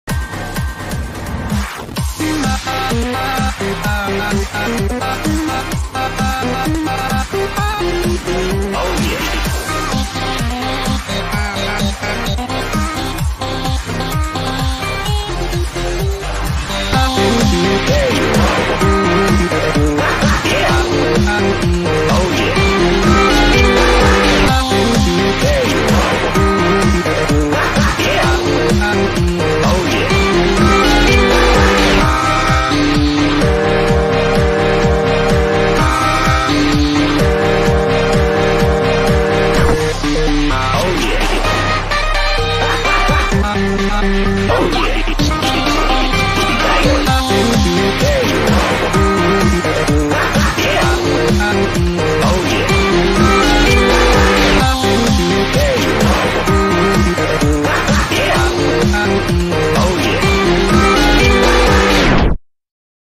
He got moves tho
Just an animation of my new oc
No, he is not a persona, my other oc is.
He’s just an oc